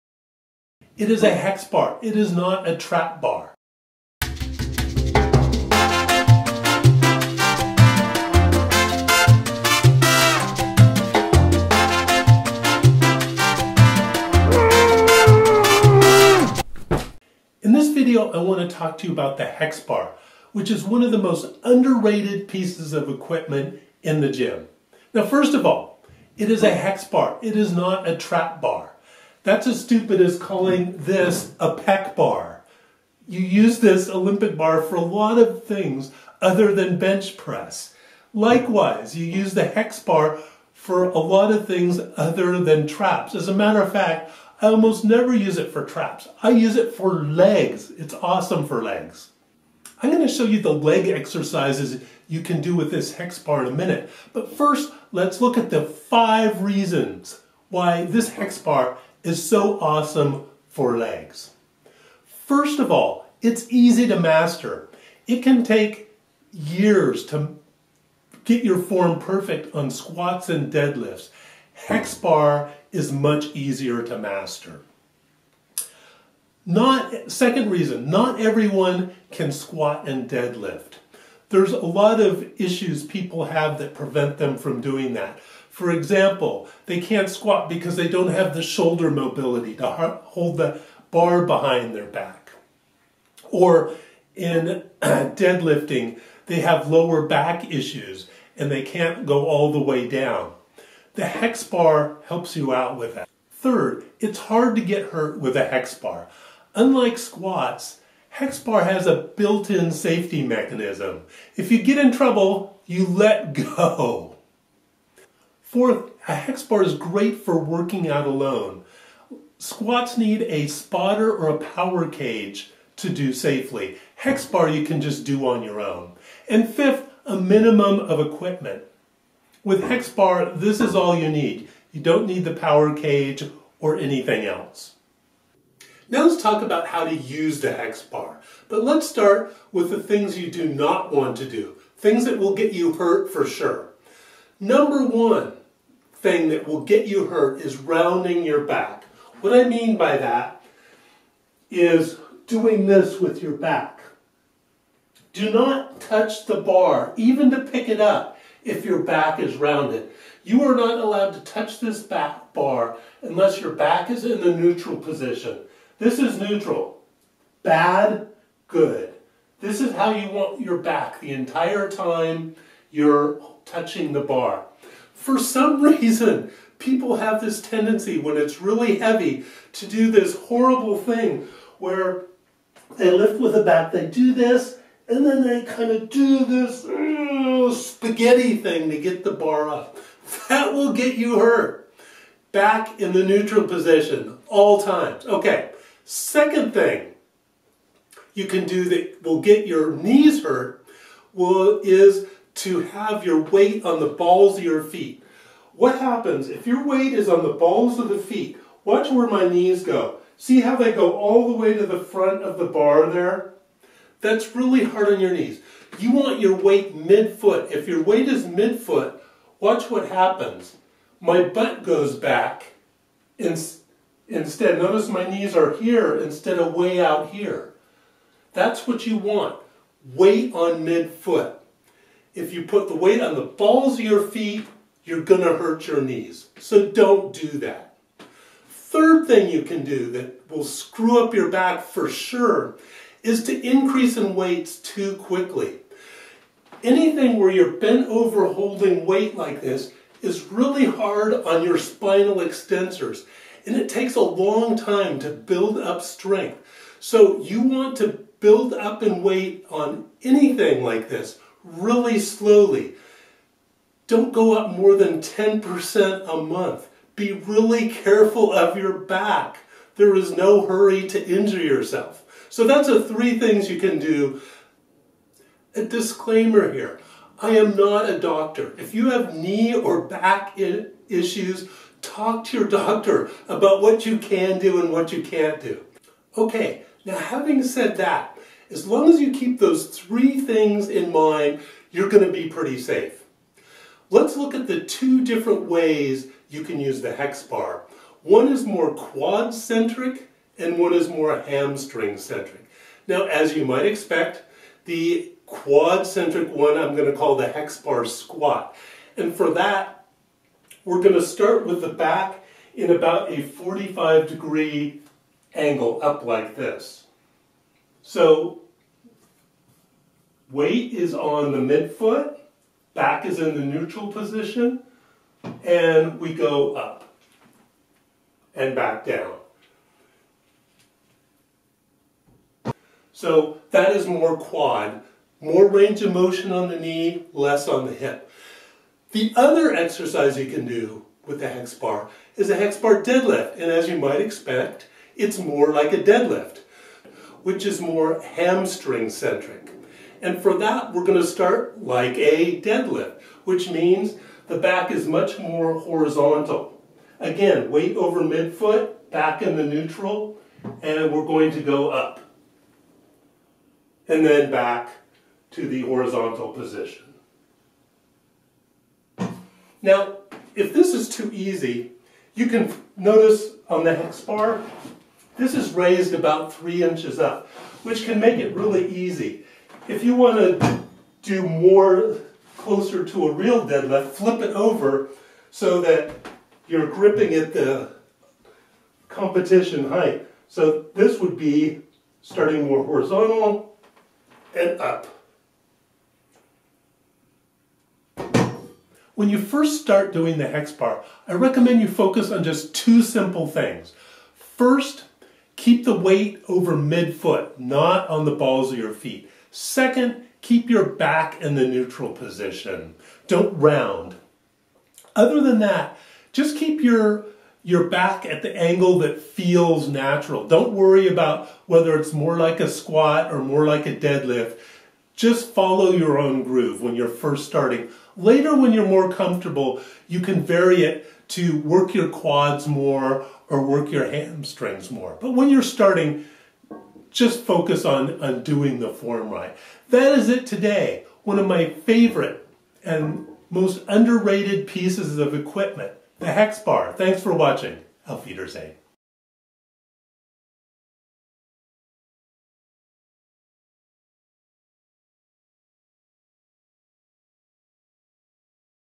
Hexbar, most under-rated gym equipment
Why are comments off? Never use a hexbar?  You are missing out!  Awesome for legs, especially at home.